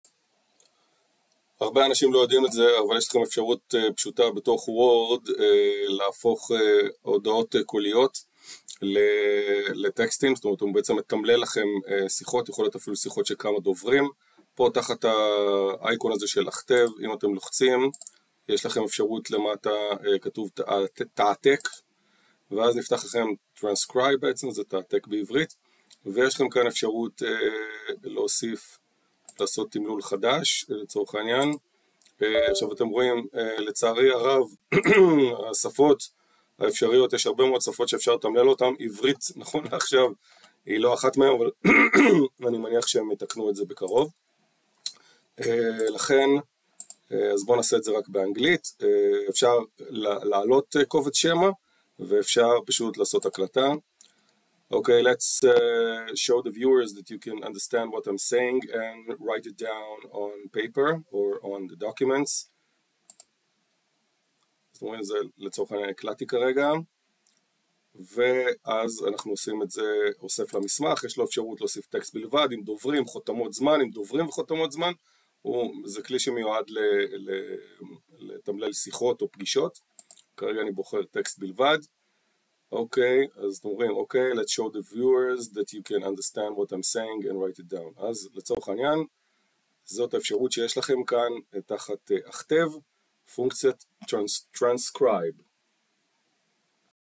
תמלול הקלטה לטקסט בוורד ובחינם
מישהו השאיר לכם הקלטה ארוכה או שיש לכם שיחה ארוכה לתמלל ואין לכם כח?
אז תתפלאו אבל יש לכם כלי לתמלל את השיחה הזו בוורד ובחינם. ראו בוידאו כיצד להשתמש בפונקציית תעתק.
לצערנו, עדיין לא זמינה בעברית, אבל עובדת כבר במלא שפות כולל אנגלית כמובן ובקרוב מניח שגם יוסיפו עברית אז שווה להכיר ולעקוב.